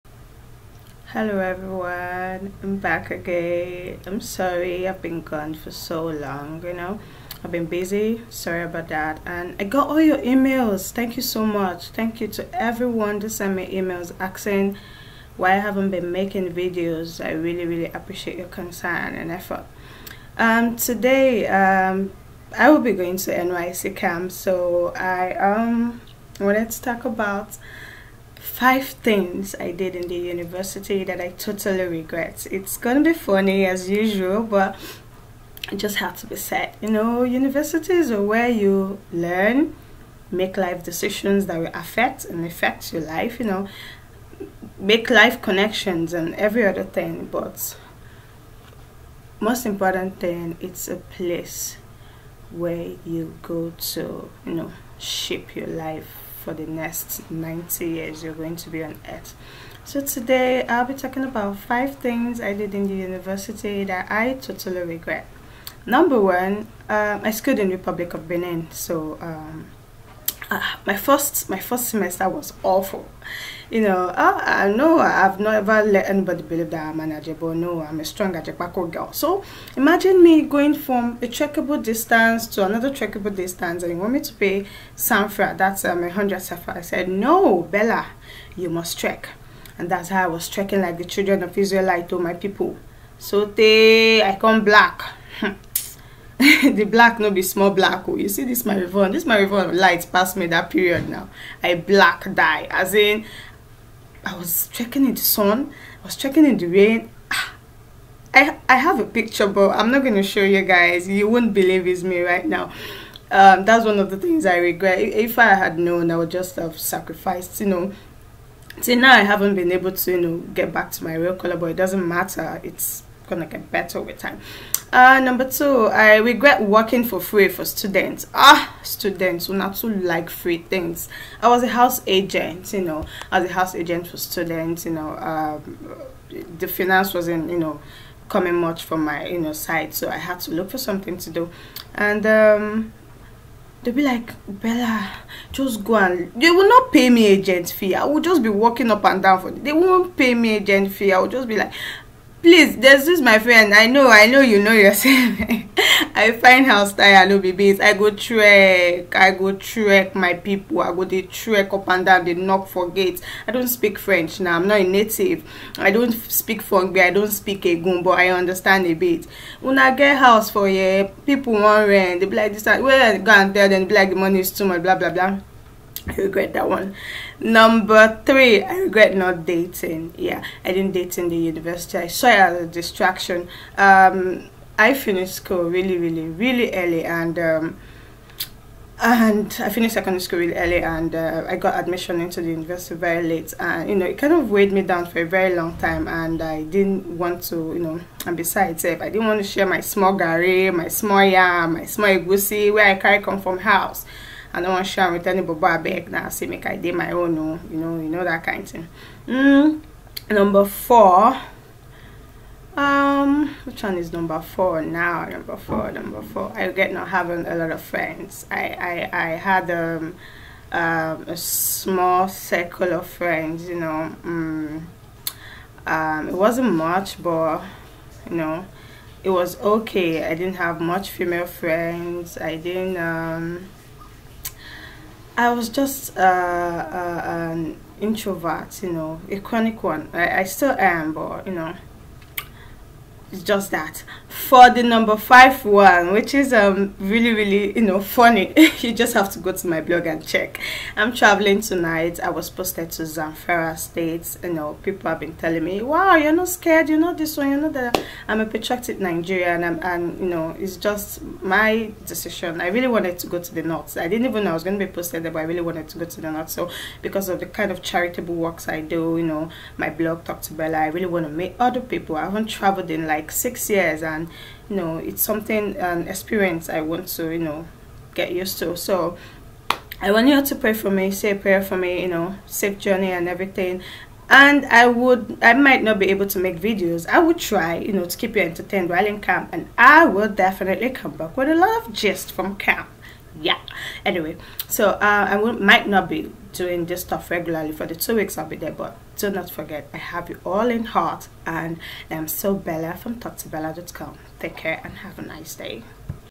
5 things i regret doing at the university
Not paying for food or paying less than I ate, mehn I no de carry last, I just eat, stand up and walk away